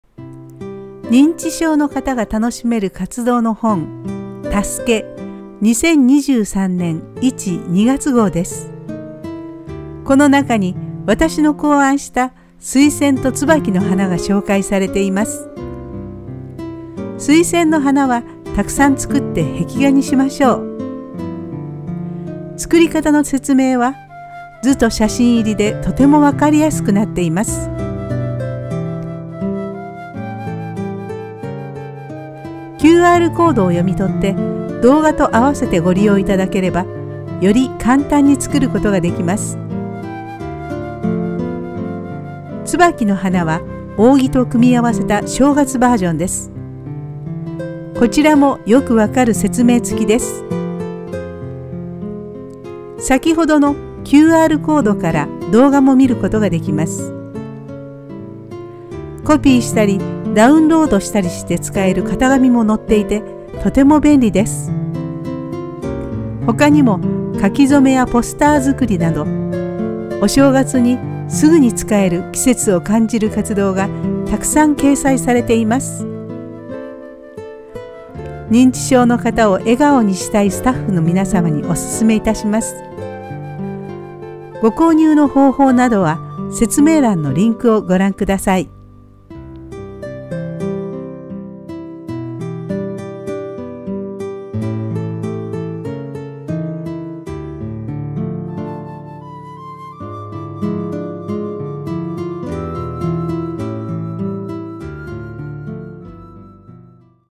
QOLサービス tasuke(たすけ)1･2号「お花紙 水仙 椿」by kimie gangi
2022年12月５日、ＱＯＬサービス出版から認知症の方が楽しめる活動の本第5弾、2023年1･2月号が発売されました。
16ページからは、私のYouTube動画を元に、お花紙を使った「季節を彩る壁画　水仙」が、28ページからは椿の花をあしらった「開運扇」が掲載されています。
写真や図を使っての詳しい説明や型紙も載っていてすぐに作ることができます。
詳しい作り方は、誌面のQRコードからご覧になれます。（限定公開の動画が含まれていますので、SNS拡散はご遠慮ください。）
「tasuke」のサイトはこちらをご覧ください。
QOLサービス出版のホームページはこちらをご覧ください。